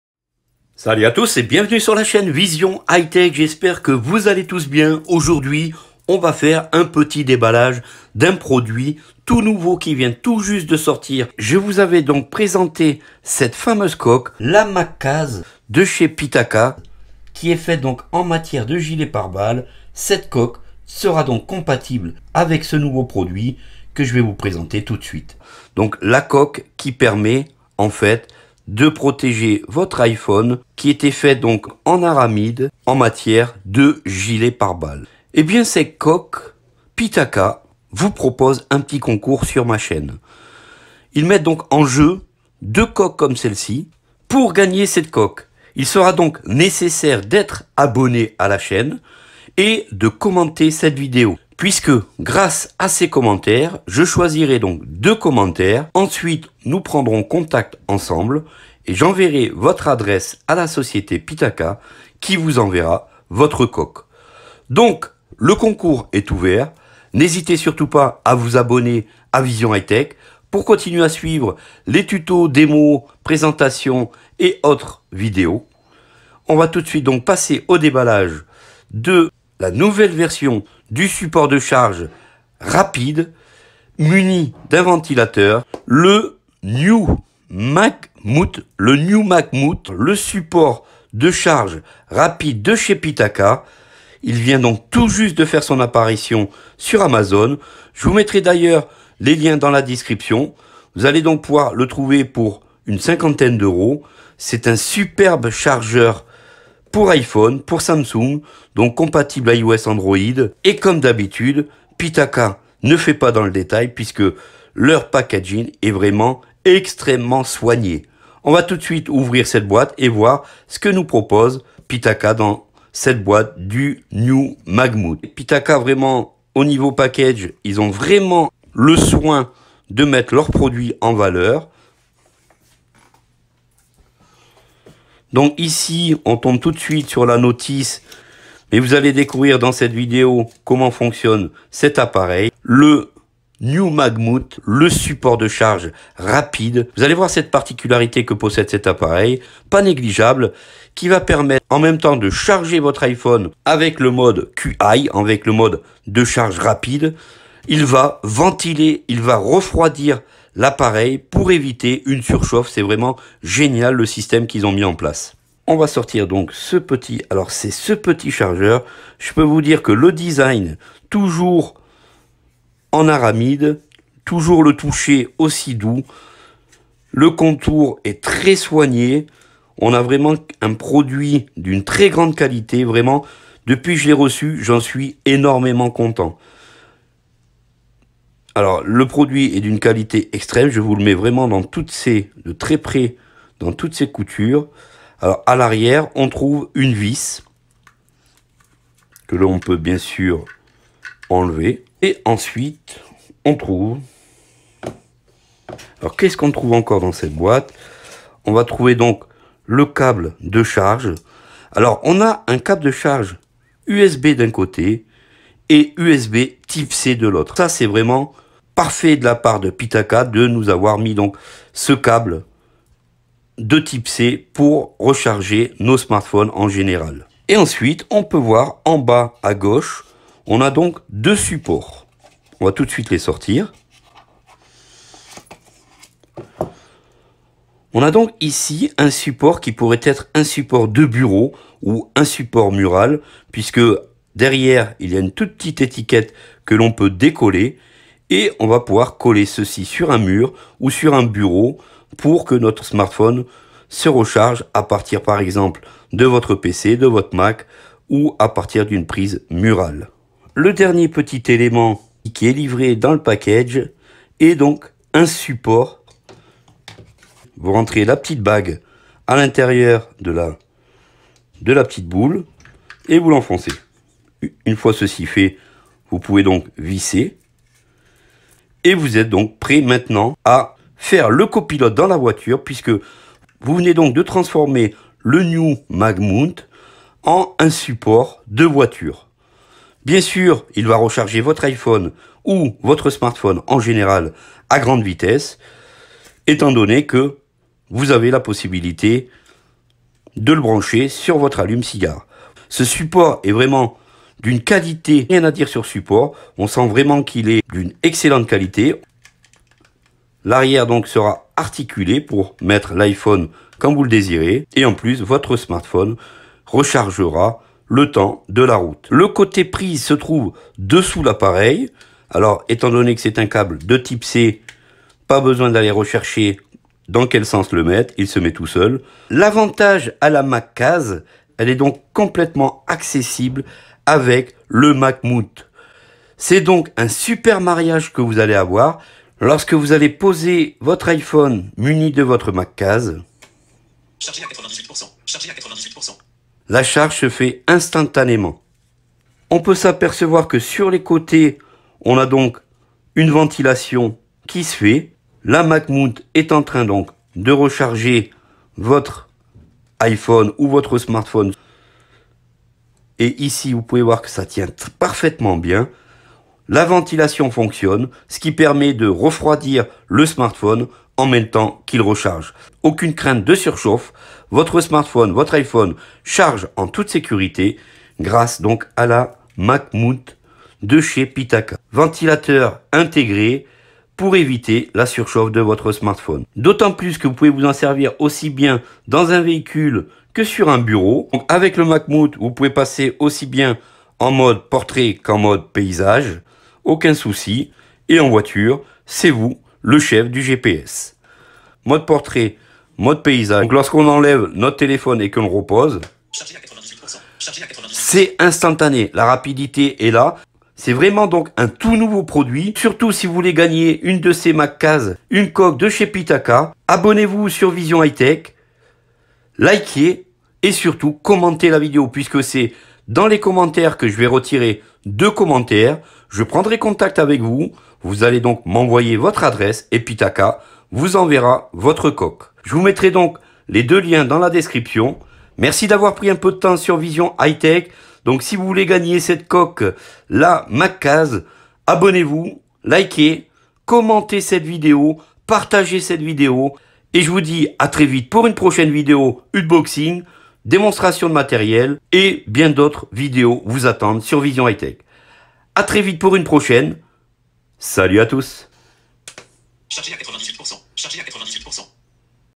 Unboxing News MagMount Chargeur Support Pitaka Chargeur Rapide Qi iPhone Androïd Grande Nouveauté !
Salut dans cette vidéo Unboxing Nouveau Support MagMount Pitaka 2 Coques  MacCase à gagner d'une valeur de 50€ organisé par Pitaka